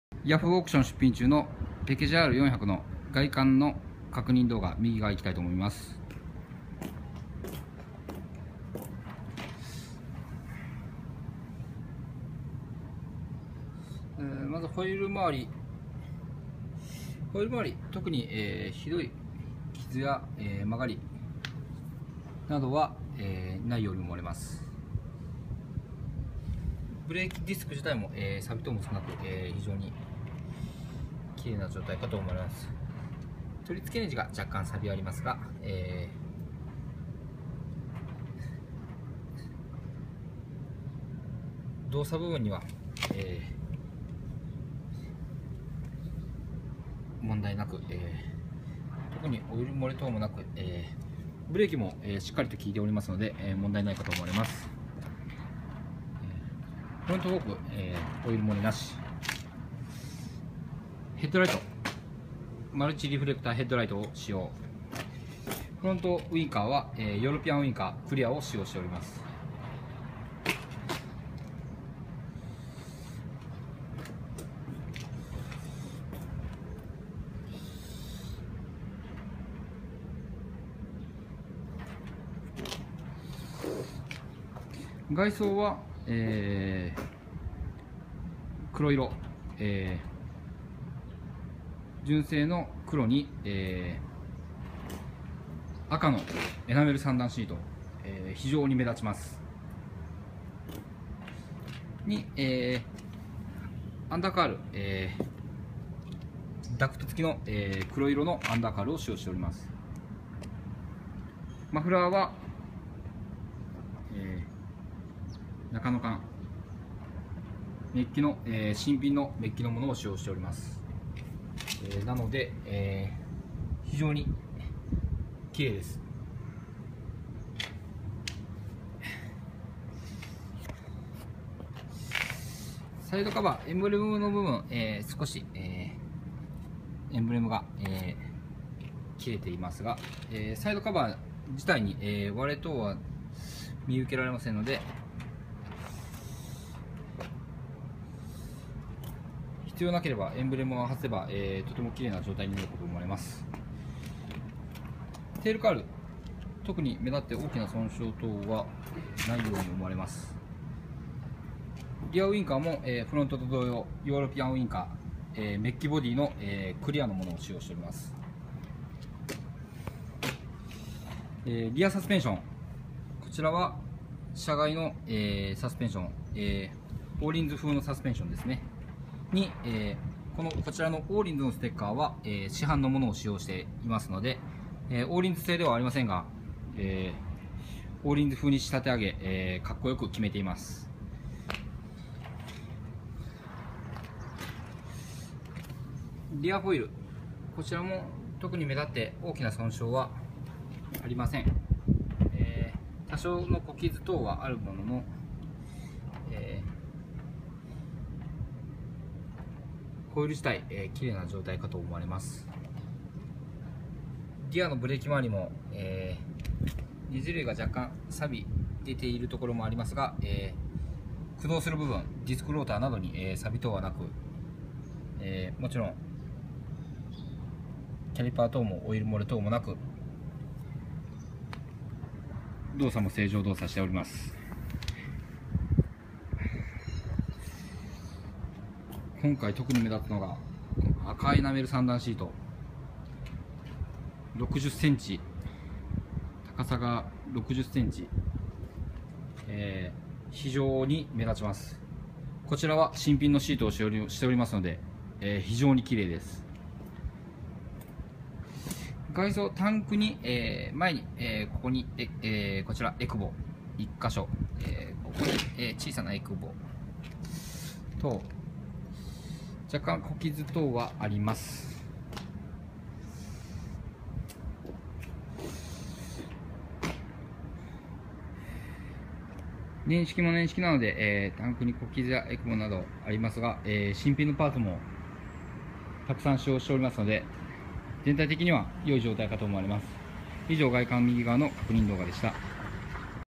XJR400 外観右側確認用 4HM-07552～
ヤフオク出品中のＸＪＲ４００の外観右側確認用の動画となります。